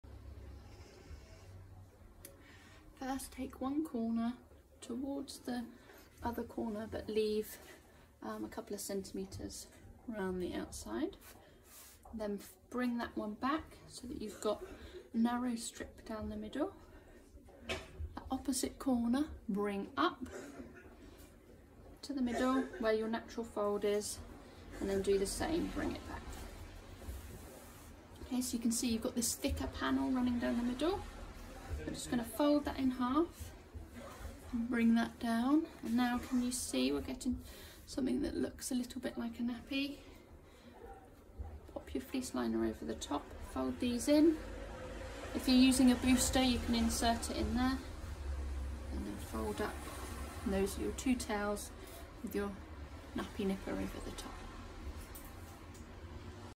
Newborn flat nappy easy fold
Using a Muslinz muslin to fold ready for use on a newborn baby. An adaptation of the Gaynors fold. Can be boosted for a bigger baby if needed. Secure with a nappy nippa and cover with a waterproof wrap.

for more support or pop into our High Street Shop in Wells, Somerset.